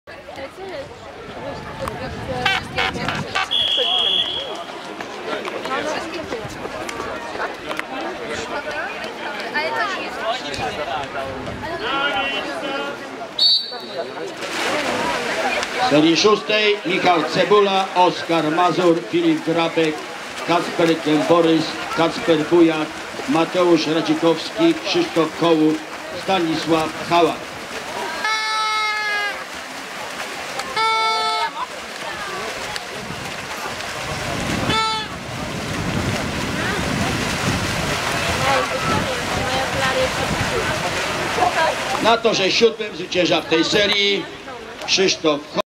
Michal C. 50 dowolny Mistrzostwa Wojewodztwa Lubelskiego Pulawy 22-23.6.2013
Skład serii: Cebula Michał 99 Sparta Biłgoraj, Mazur Oskar 99 Fala Kraśnik, Grabek Filip 99 Lublinianka, Gęborys Kacper 98 AWF Biała Podlaska, Bujak Kacper 98 Lublinianka, Radzikowski Mateusz 99 Lublinianka, Hołub Krzysztof 00 Avia Świdnik, Chałat Stanisław 99 Skarpa Lublin